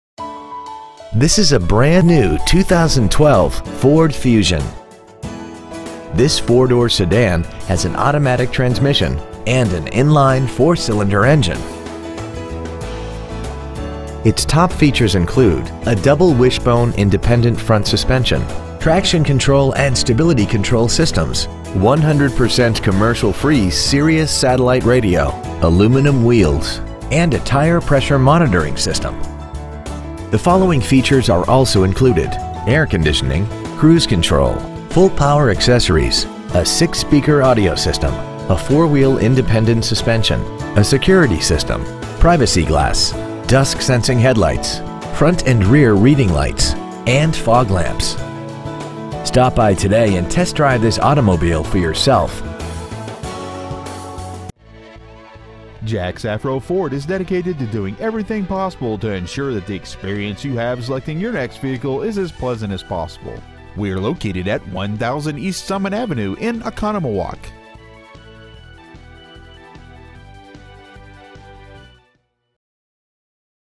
2012 Ford Fusion Oconomowoc WI
We have been honored to serve the Oconomowoc WI area , we promise that your experience at our dealership will exceed your expectations !

 Year : 2012
 Make : Ford
 Model : Fusion
 Engine : 2.5L I4 16V MPFI DOHC
 Trans . : AUTOMATIC
 Exterior : CINNAMON METALLIC
 Miles : 1
 Interior : CAMEL
 Stock : 126558